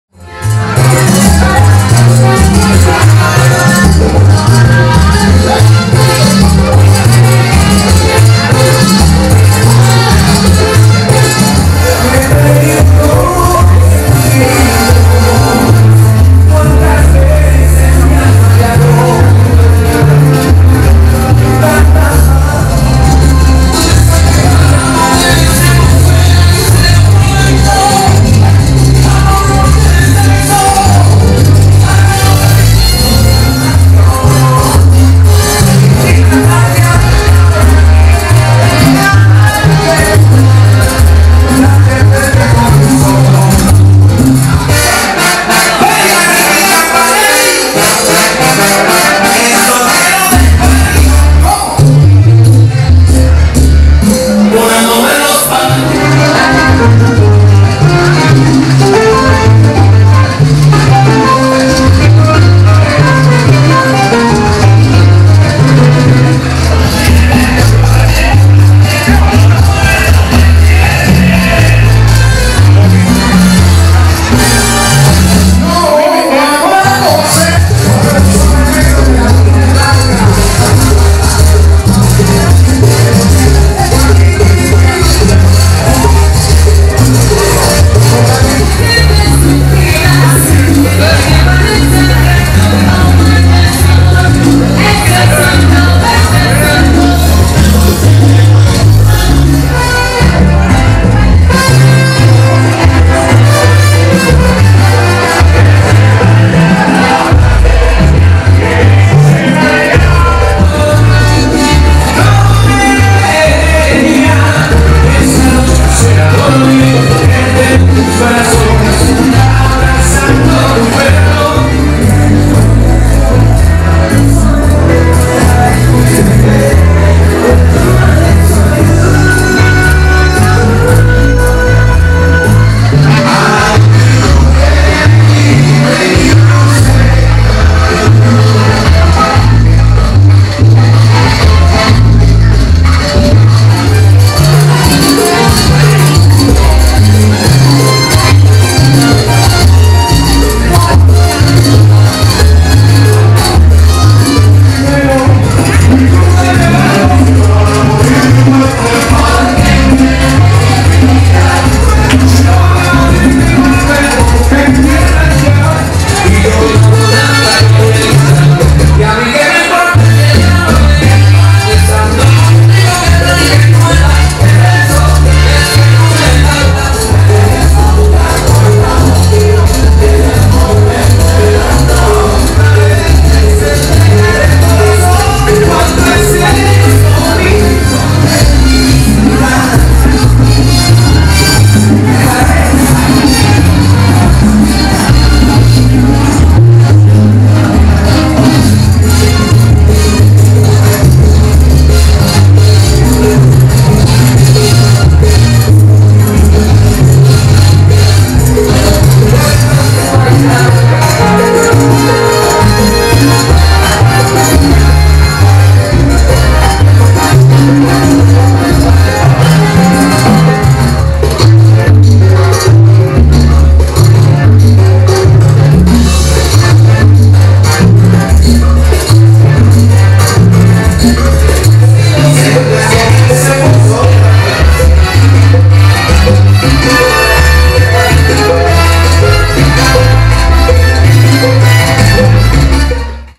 Smooth Salsa UK Brook Street January 7th 2023
Smooth started 2023 with a great party as always plenty of dancers came out to dance at the event held at The Brook Street Community Centre. The DJs music was on point the floor stayed busy until the end of the night, don't believe me? watch the clips and see.

Smooth Salsa parties are held monthly.